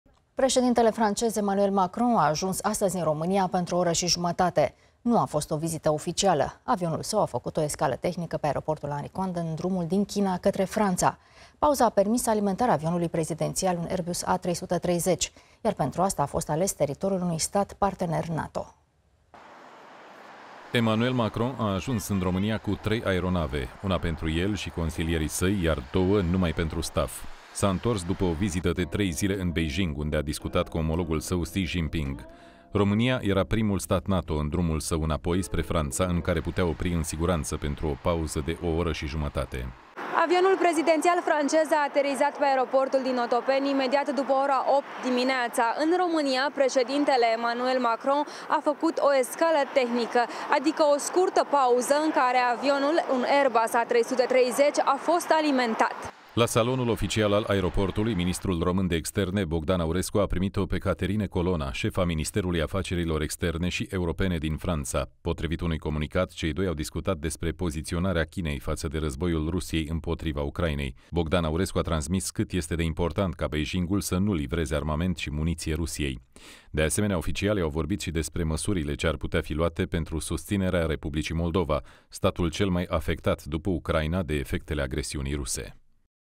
Emmanuel Macron a ajuns în România, după vizita din China
Președintele francez Emmanuel Macron a ajuns, sâmbătă, în România, pentru o oră și jumătate. Nu a fost o vizită oficială. Avionul său a făcut o escală tehnică pe aeroportul Henri Coandă, în drumul din China către Franța.